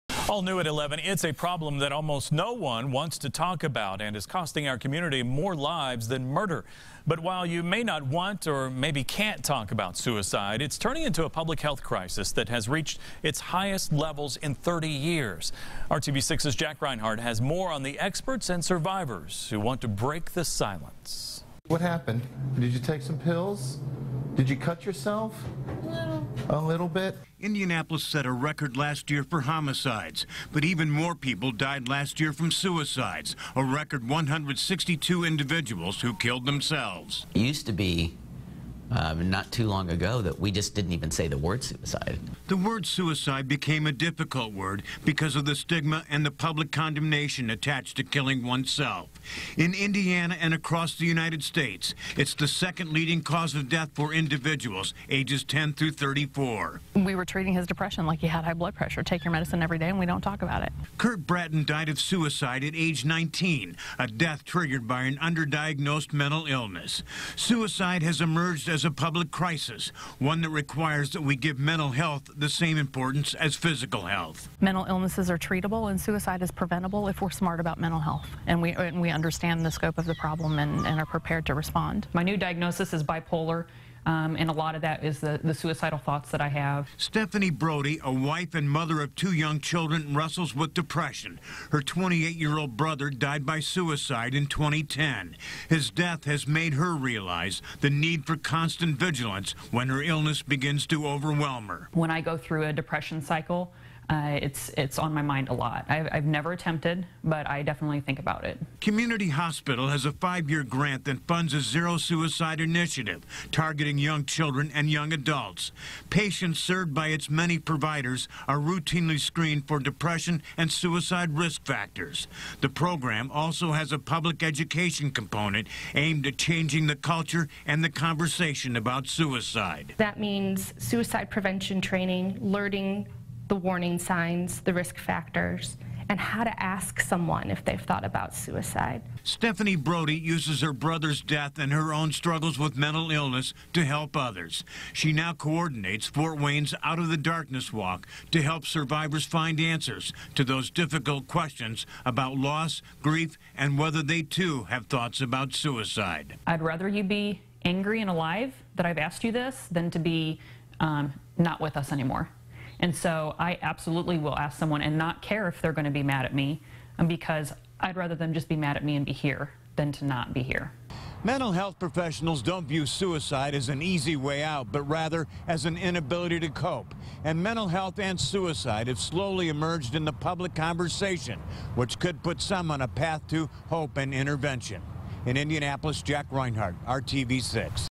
Suicides take more lives in Indiana than homicides
Suicide is second-leading cause of death among 10-to-34-year-olds